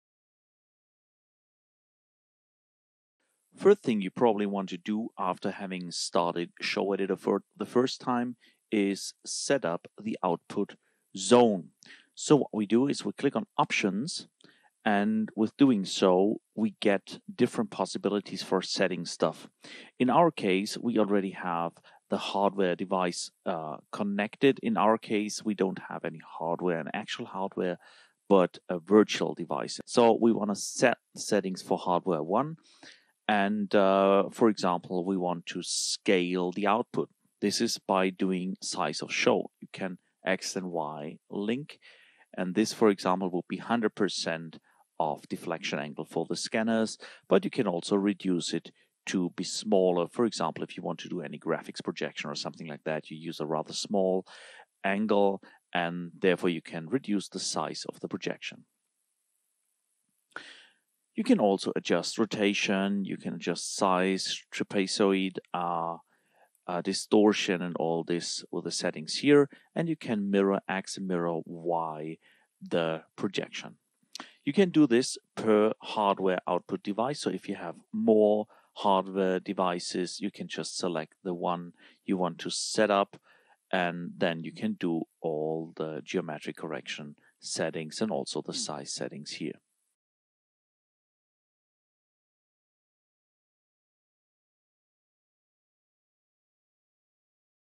How to set the Projection Zone in Showeditor
This tutorial describes ho the projection zone can be set in Showeditor